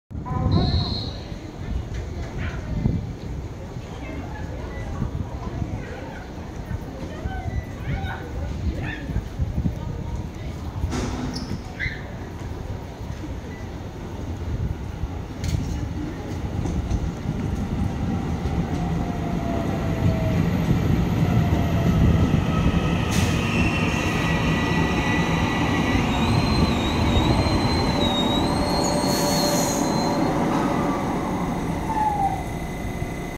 臺鐵1231次區間車中壢站進站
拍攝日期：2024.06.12
拍攝車次：1231
行駛區間：基隆--》新竹
拍攝編組：EMU930